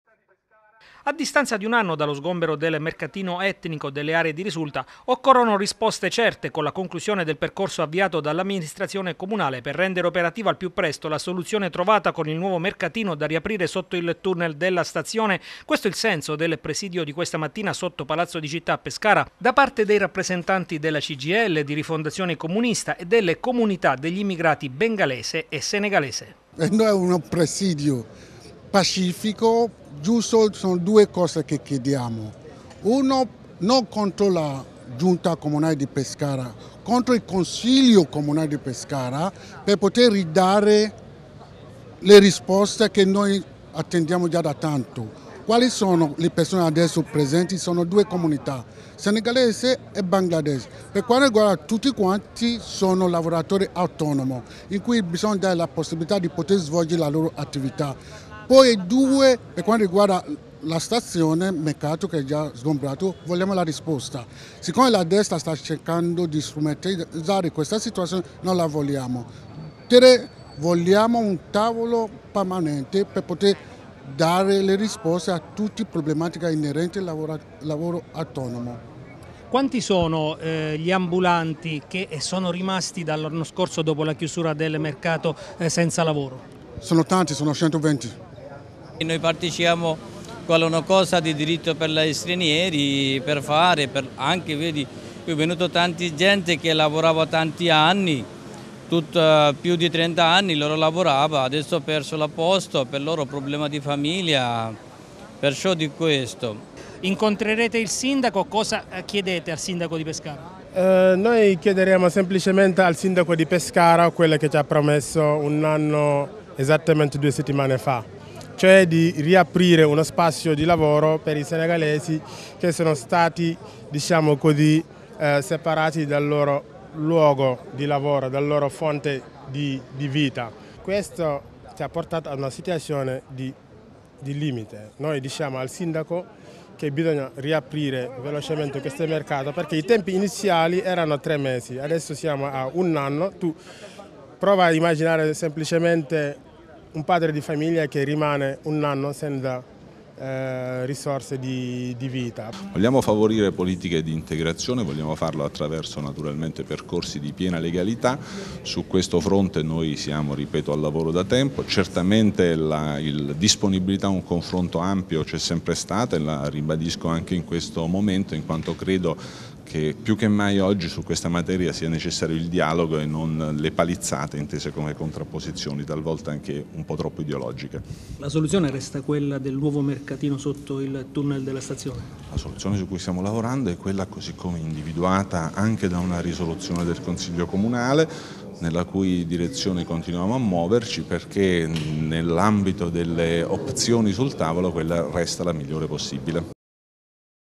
Nuovo mercatino etnico a Pescara: Presidio davanti Palazzo di Citta
Presidio di Cgil, Rifondazione Comunista e rappresentanza di immigrati delle comunità del Senegal e bengalese questa mattina a Pescara per chiedere l'apertura del nuovo mercatino etnico. Successivamente c'è stato l'incontro con il sindaco Marco Alessandrini